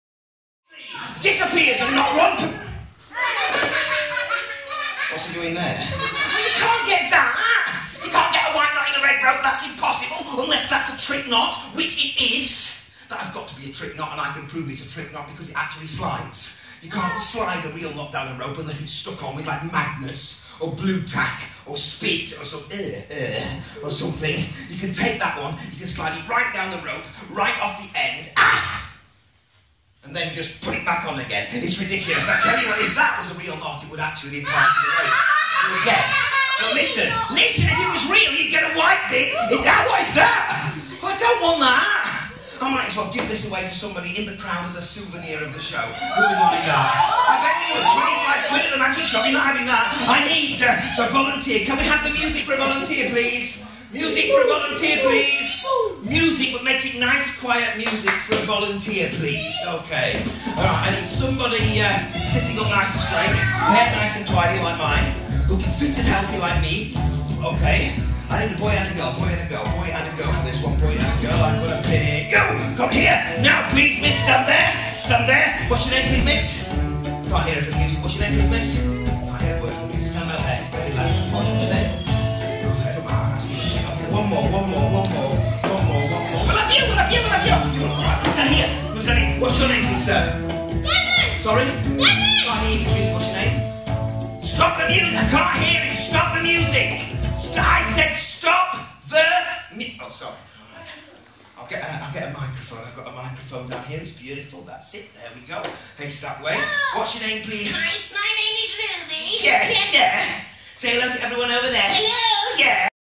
SESKU Holiday Club Dec 2014 Kimmo (Part 2)
December 2014 SESKU Academy Holiday Club KimmoChildren's Entertainer Magic, Ventriloquism and Balloon Animals.